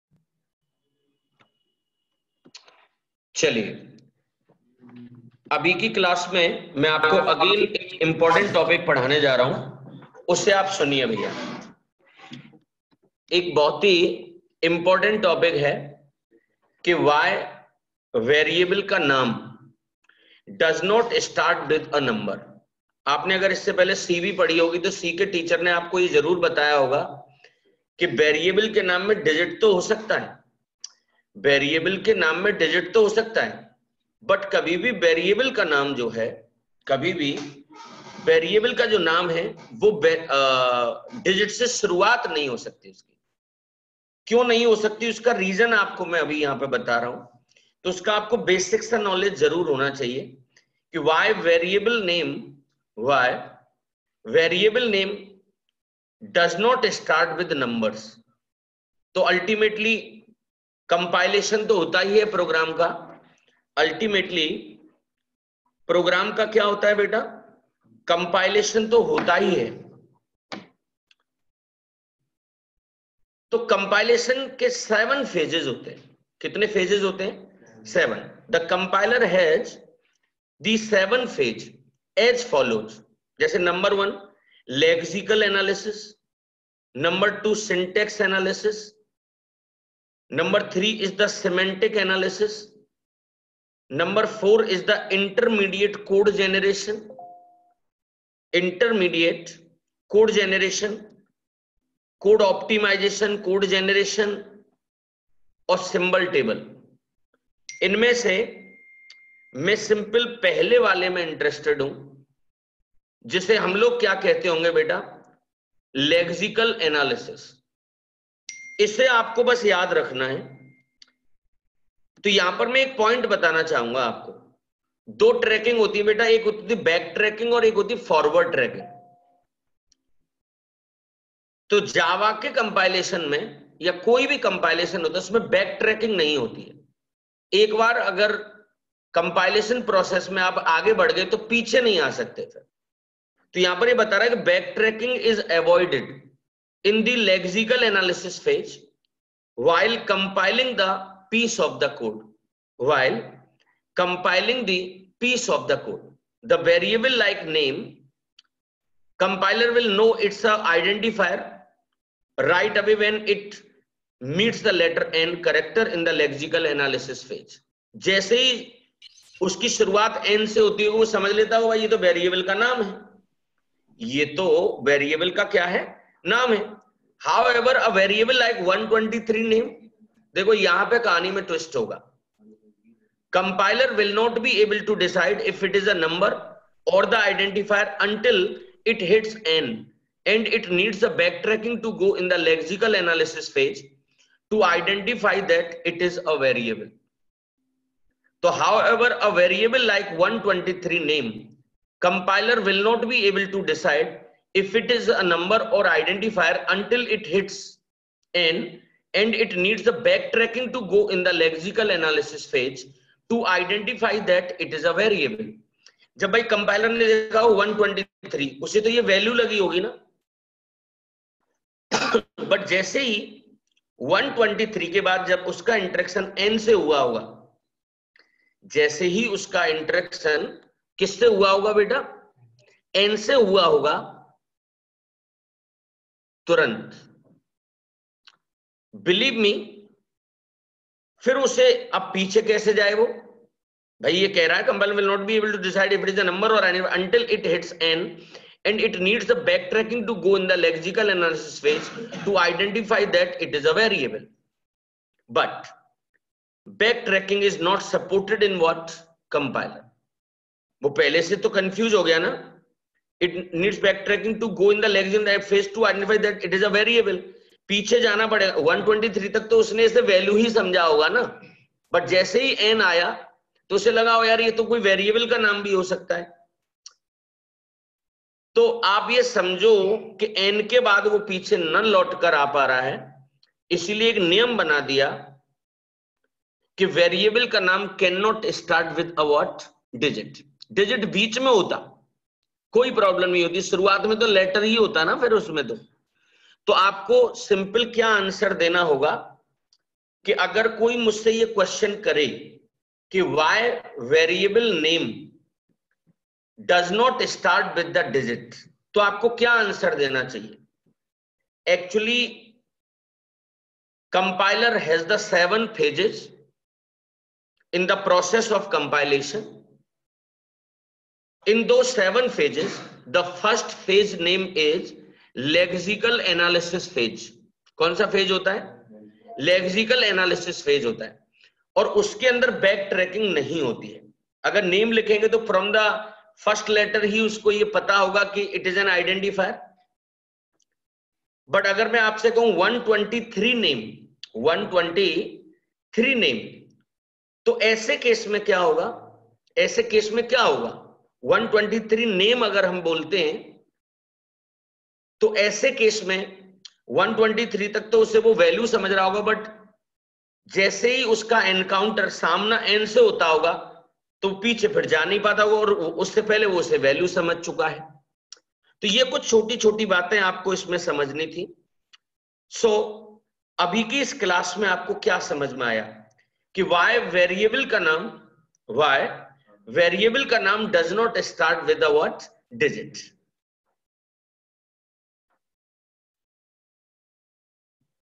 13- Why Variable Name Does Not Start With a Digit || Classroom Recorded Video By DD Singh
Hello Friends in this live session i discussed about Why_Variable_Name_Does_Not_Start_With_a_Digit in java.